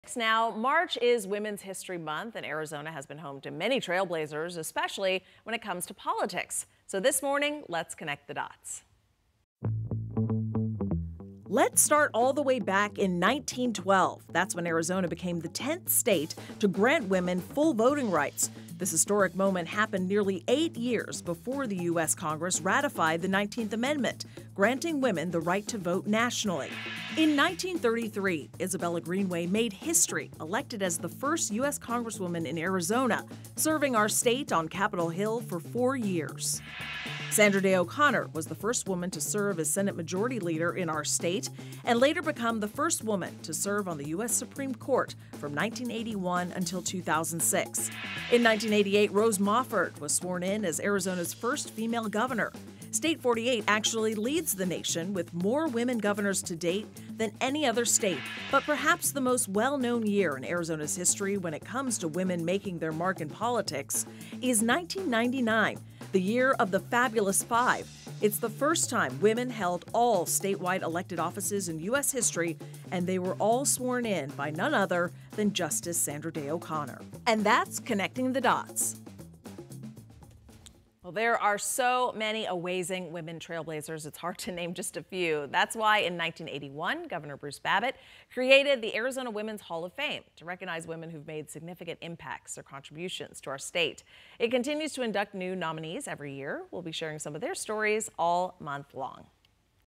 Connect The Dots: Women's History Month
What is Arizona's role in Women's History Month? We're connecting the dots.